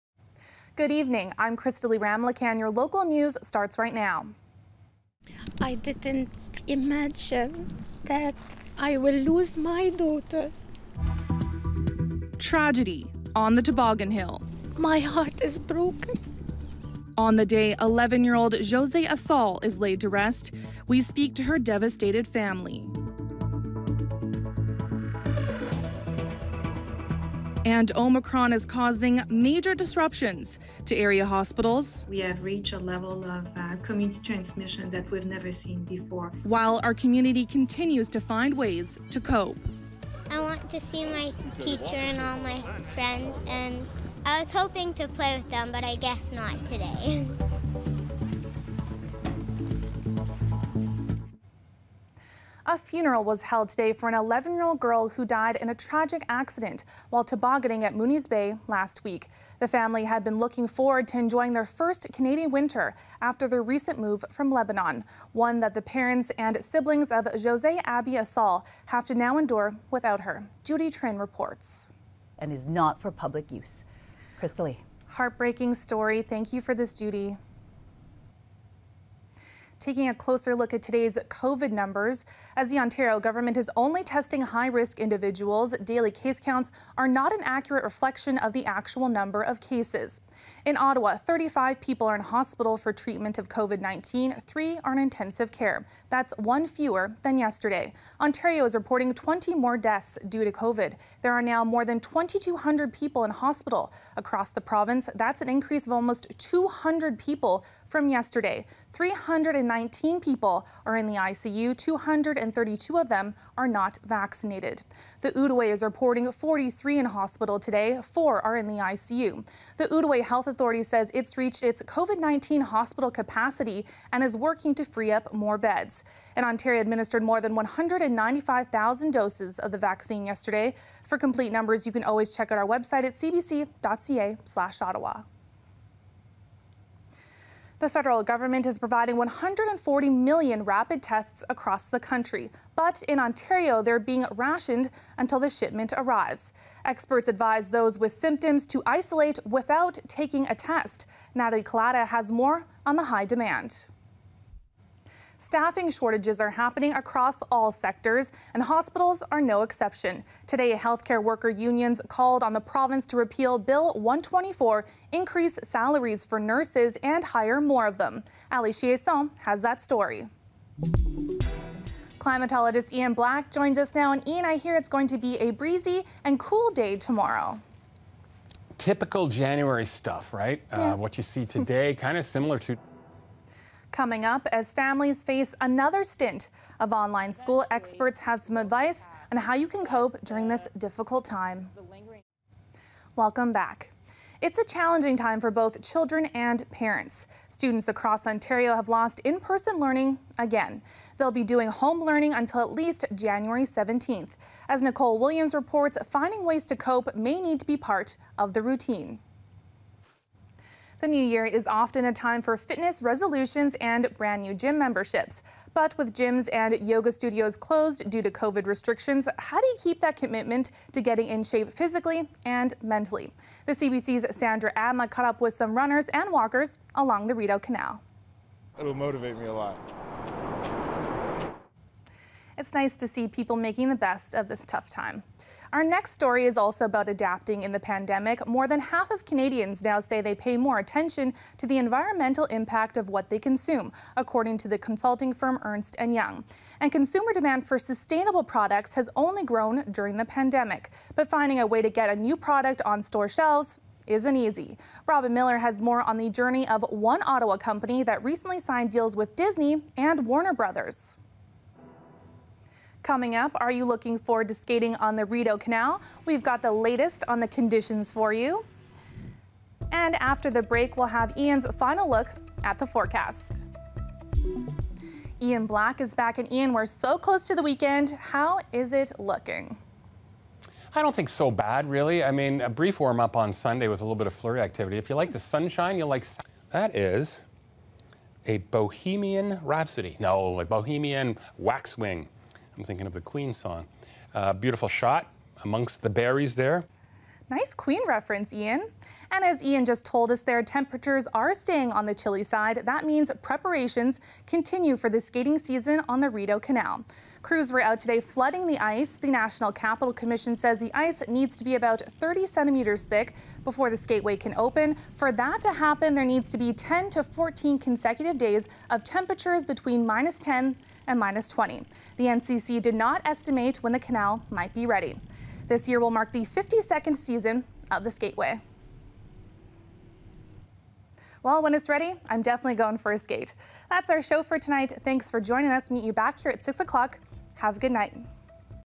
Live newscast hosted by Krystalle Ramlakhan for CBC Ottawa
Krystalle Ramlakhan produced and hosted this newcast for CBC Ottawa on January 6, 2022.